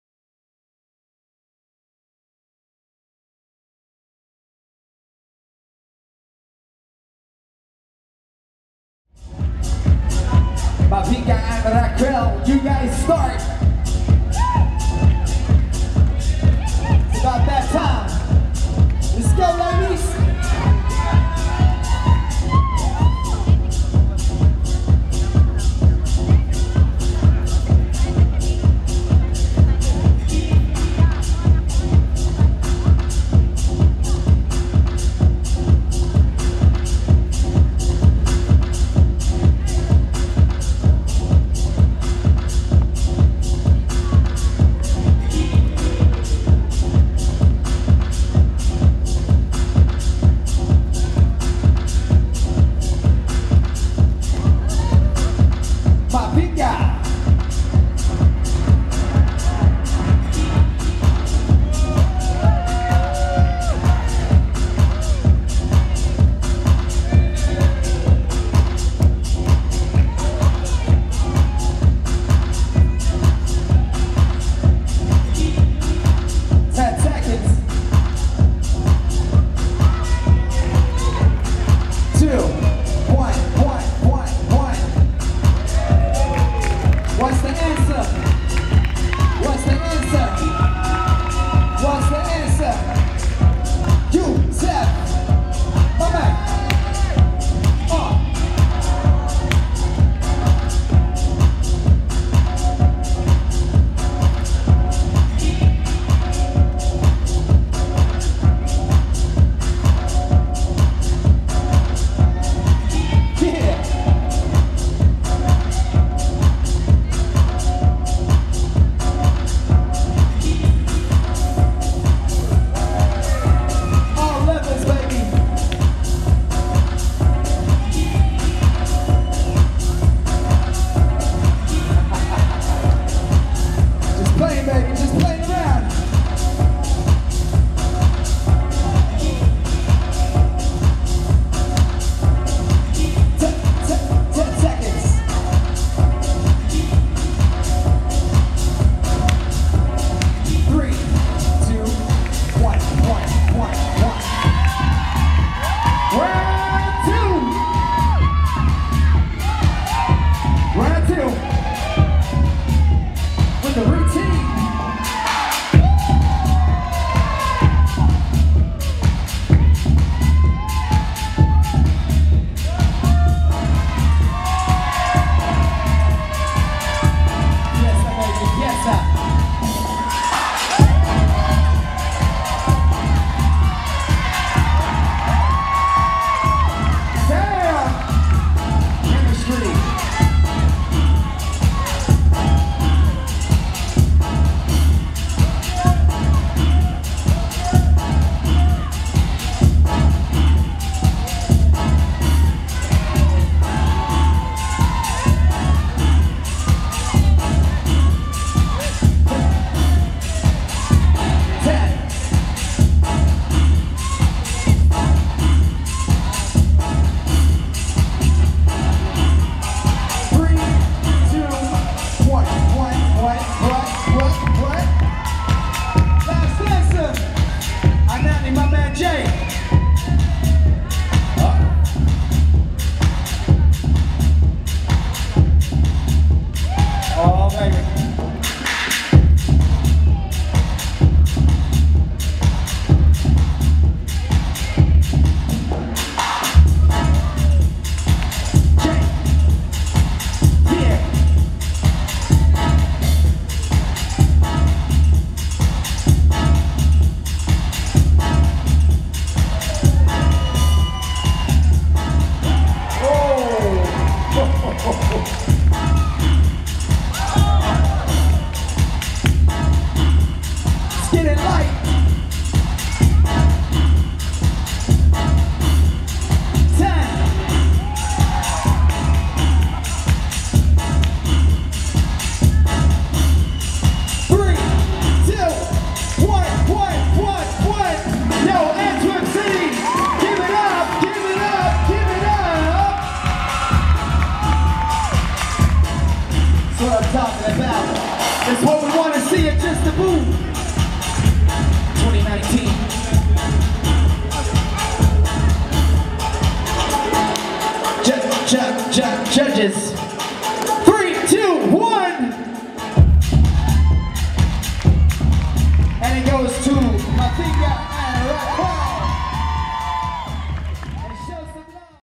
Juste Debout Antwerp 2019 | House 1:4 Final IV
House 1/4 Final IV
Mavinga & Raquel (BEL) vs Anané & Youssef (BEL)
Juste Debout Antwerp, Belgium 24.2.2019
Urban Center Antwerp
Judges / Juste Debout: Manu (COG/FRA), Nelson (FRA), Tony Mc Gregor (USA), TweetBoogie (USA)
Judges / Junior Dance Tour: Bboy Bojin (TPE), Funky J (FRA), Alex The Cage (BEL)
DJs: DJ Raf (RUS) & DJ Bironnex (FRA)
MC: MC Sambo (BEL)
Video: Ocloo Productions